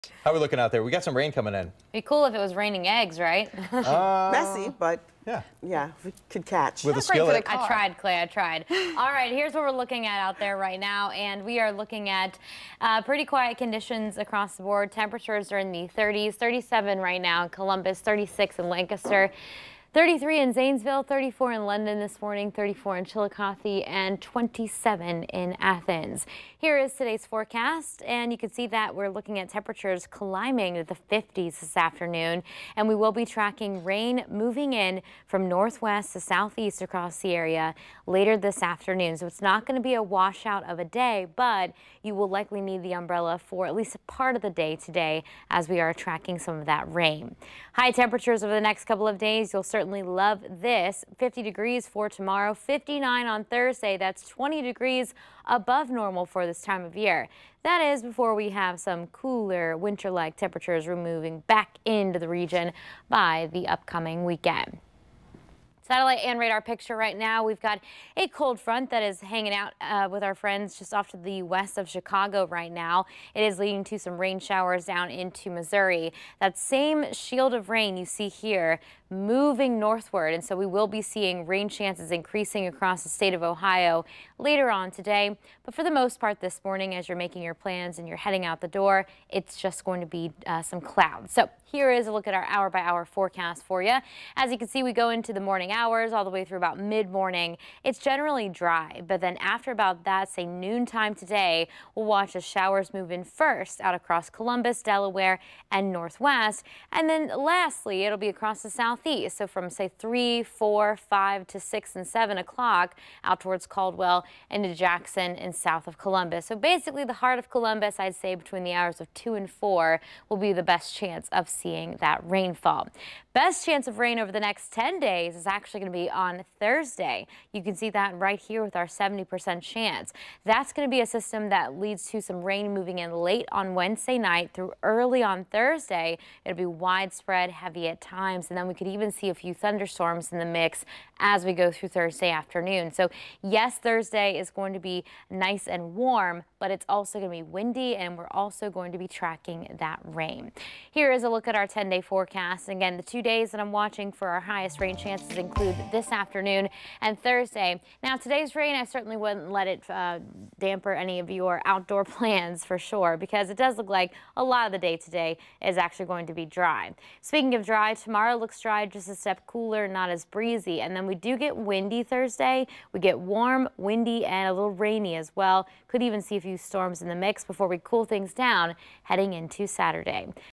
Columbus, Ohio weather forecast | Feb. 7, 2023
Today: Cloudy and breezy with scattered rain during the afternoon. High 54. Winds: S 10-20 mph